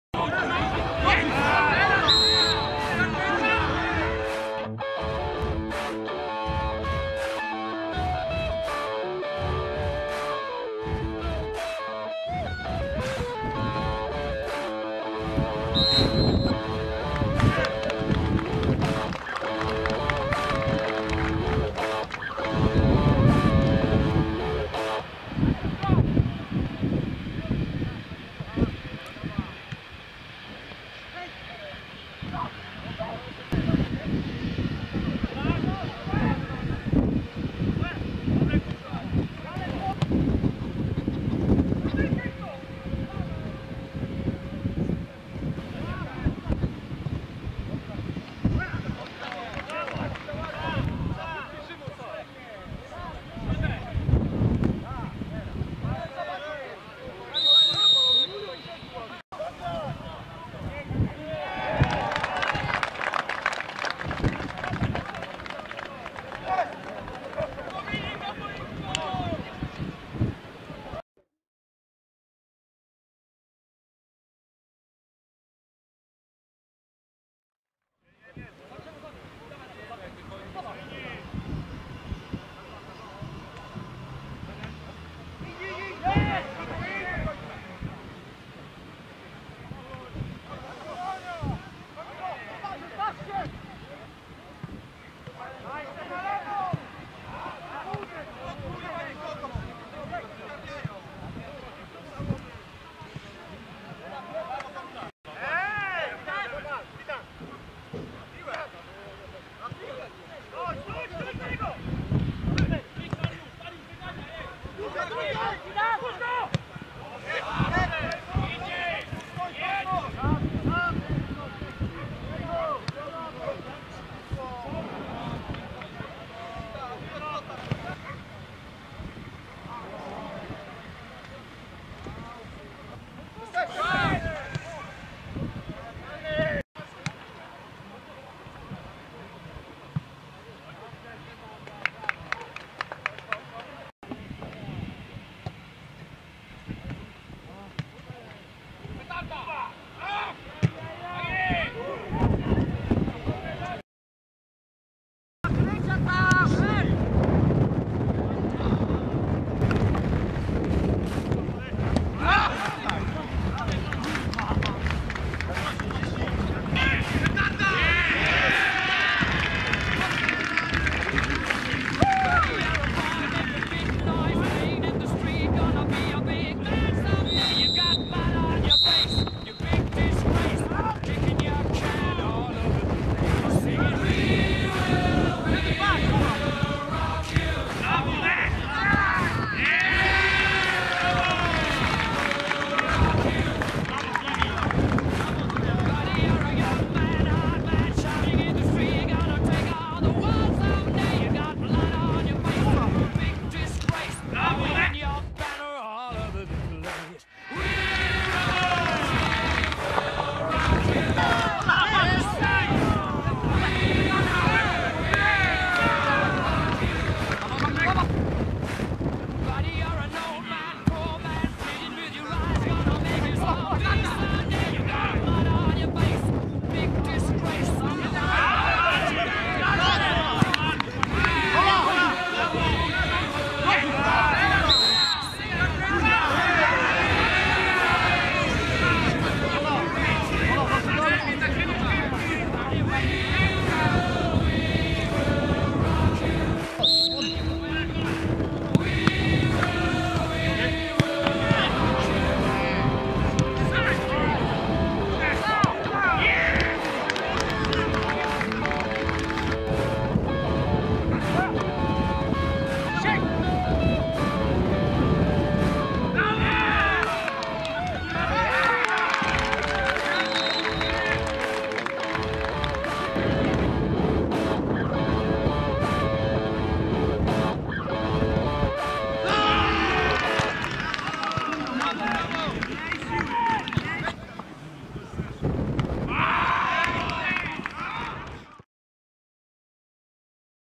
CKS Zbiersk - Czarni Wierzbno [Skrót]
[12. kolejka A-klasy, Kalisz] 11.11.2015, stadion w Zbiersku

Spotkanie ligowe z drużyną Czarnych Wierzbno zakończyło się zwycięstwem CKS-u Zbiersk 4:1
Po dwie bramki zdobyli D. Bangrowski oraz D. Drzewiecki.